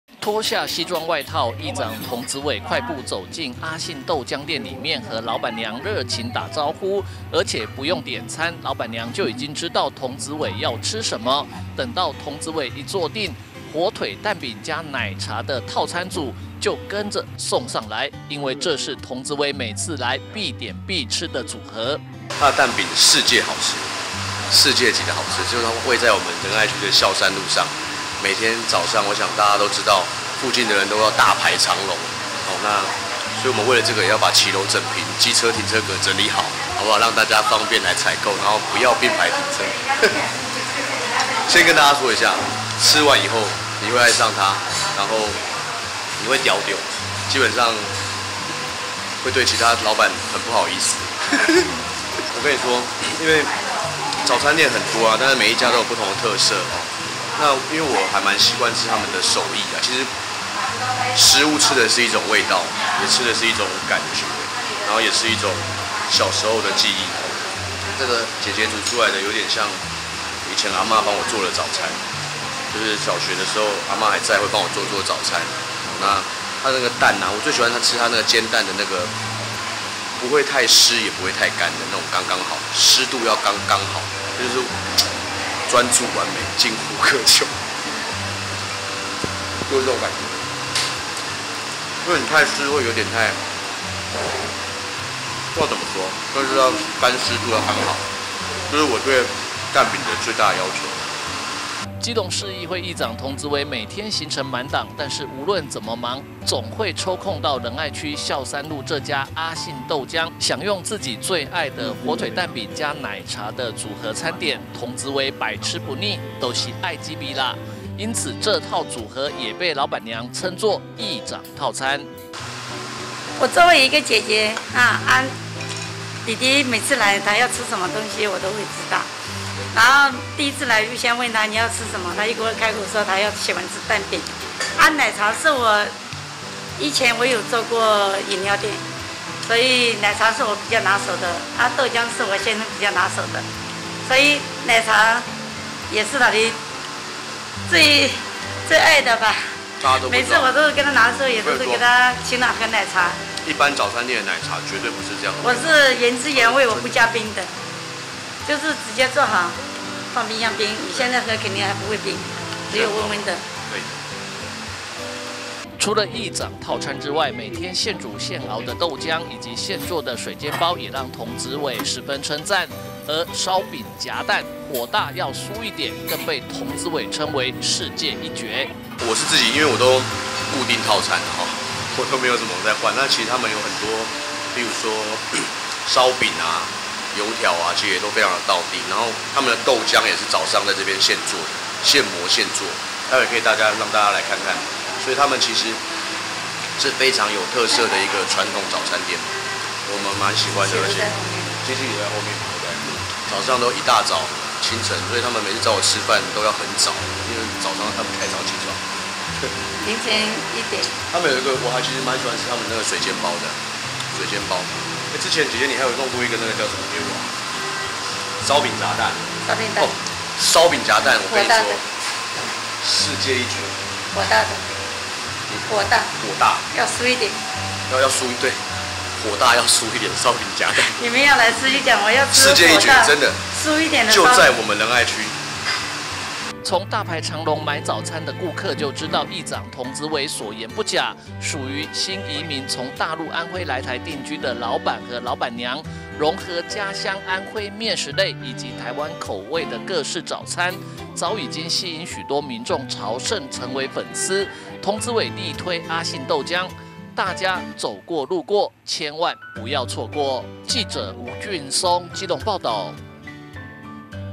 基隆|童子瑋|阿信豆漿|童子瑋乾姊|火腿蛋餅|奶茶|燒餅夾蛋|水煎包|豆漿|油條|議長套餐|孝三路|阿信豆漿火腿蛋餅加奶茶 議長套餐童子瑋最愛/「燒餅夾蛋」火大酥一點 童子瑋讚「世界一絕」
阿信豆漿火腿蛋餅加奶茶 議長套餐童子瑋最愛

「燒餅夾蛋」火大酥一點 童子瑋讚「世界一絕」

(中嘉吉隆新聞記者吳俊松/基隆報導)基隆市議會議長童子瑋每天行程滿檔，但是無論怎麼忙，總會抽空到仁愛區孝三路的”阿信豆漿”享用自己最愛的”火腿蛋餅加奶茶”的餐點，每次必點，因此豆漿店老闆娘特地把這項點組合稱為”議長套餐”，也深獲童子瑋的大力推薦！

脫下西裝外套，議長童子瑋快步走進阿信豆漿店裡面和老闆娘熱情打招呼，而且不用點餐，老闆娘就已經知道童子瑋要吃甚麼，等到童子瑋一坐定，火腿蛋餅加奶茶的套餐組，就已經送上來，因為這是童子瑋每次來必點必吃的組合！

基隆市議會議長童子瑋每天行程滿檔，但是無論怎麼忙，總會抽空到仁愛區孝三路這家”阿信豆漿”享用自己最愛的”火腿蛋餅加奶茶”的組合餐點，童子瑋百吃不膩，就是愛這味啦，因此這套組合也被老闆娘稱做”議長套餐”！

除了”議長套餐”之外，每天現煮現熬的豆漿以及現做的水煎包也讓童子瑋十分稱讚，而”燒餅夾蛋”，”火大要酥一點”更被童子瑋稱為”世界一絕”！

從大排長龍買早餐的顧客就知道議長童子瑋所言不假，屬於新移民從大陸安徽來台定居的老闆和老闆娘，融合家鄉安徽麵食類以及台灣口味的各式早餐，早已經吸引許多民眾朝聖成為粉絲，童子瑋力推”阿信豆漿”，大家走過路過千萬不要錯過！

店名:阿信豆漿

地址:基隆市仁愛區孝三路75號